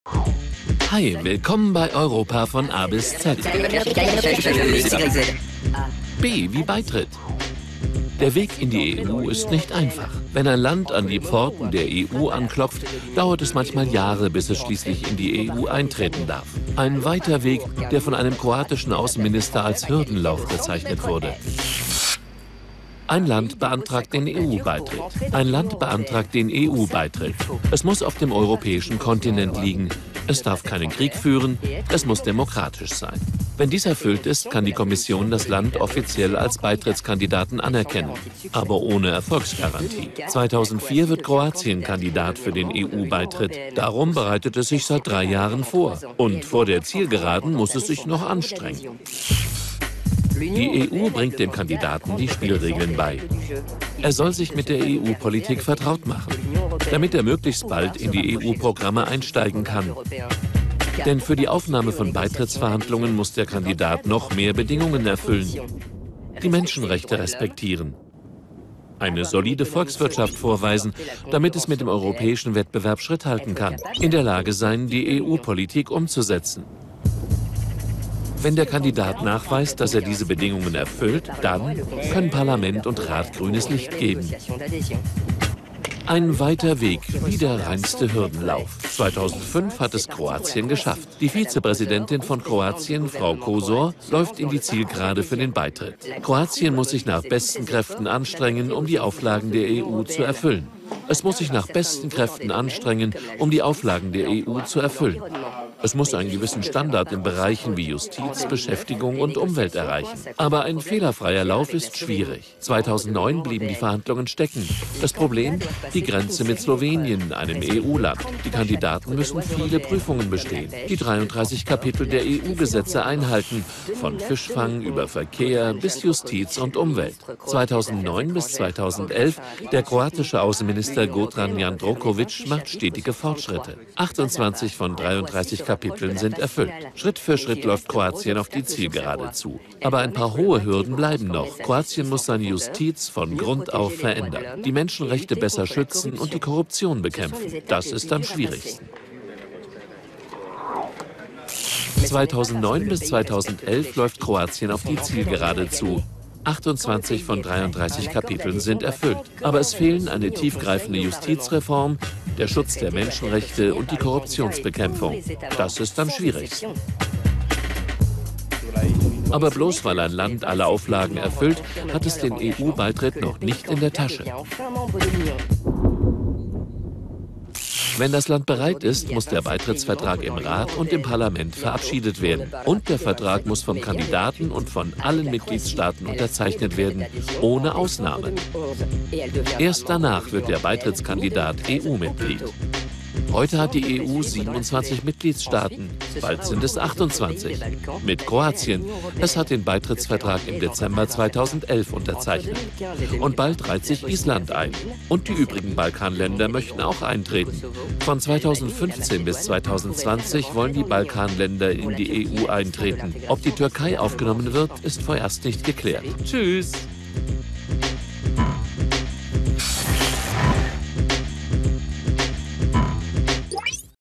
A-Z: A for Accession
Hi! Welcome to A-Z. A-B-C-D... A for Accession. It's not easy to join the EU. Between the application date and the entry date, it can take several years. The Croatian Foreign Affairs Minister said it was like the 110 metres hurdles. A country has to apply to join the EU. For that, the country must be on the European continent, not be at war and be a democracy. Only then can the Commission declare it to be an official candidate, but with no guarantees of success. In 2004, Croatia becomes a candidate for EU membership, after three years of preparation. And it needs to do more before starting the accession talks. The EU helps the candidate to learn the rules of the game. The country must familiarise itself with EU policies in order to adapt to EU programmes, for to take part in the accession talks it must meet many other conditions. Respect human rights. Have a strong economy in order to withstand EU competition. And be able to apply EU policies. If the candidate meets all these conditions, the Parliament and Council may give the go-ahead to start the accession talks. A long process which resembles the 110 metres hurdles. In 2005, it's kick-off for Croatia. Croatian Vice Prime Minister Kosor starts the accession process. Croatia must make every effort to adapt to EU requirements and reach a certain level in areas like justice, employment and environment. But it's difficult to do a clear round. In 2009, the talks get stuck due to a border question with Slovenia, an EU member. Candidates must pass a number of tests: they must respect 33 chapters of EU law like Fisheries, Transport, Justice and Environment. In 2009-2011, Gordan Jandroković, Croatian Foreign Affairs Minister is making steady progress. The EU has closed 28 out of 33 chapters. Croatia is approaching the final stage, but major obstacles remain. It must radically reform its judicial system, ensure a better protection of human rights and fight corruption. These are the hardest steps to take. But implementing the chapters doesn't mean the country will enter the EU. When the country is ready, an accession agreement must be submitted to the Council and the EP. This treaty must be signed by the candidate and all the Member States. Only when the 27 Member States have ratified the agreement can the country join the EU. Today, there are 27 Member States in the EU. Soon we'll be talking about the 28 Member States, with Croatia, which signed the accession treaty in December 2011 and will become a member of the EU in mid-2013 at the earliest. Iceland should follow soon after. Then it will be the turn of the other Balkan countries. The schedule is still uncertain. Between 2015 and 2020, Bosnia and Herzegovina, Serbia, Montenegro, Kosovo, Albania and the former Yugoslav Republic of Macedonia could join the EU. As for Turkey, the integration process remains on hold. Goodbye.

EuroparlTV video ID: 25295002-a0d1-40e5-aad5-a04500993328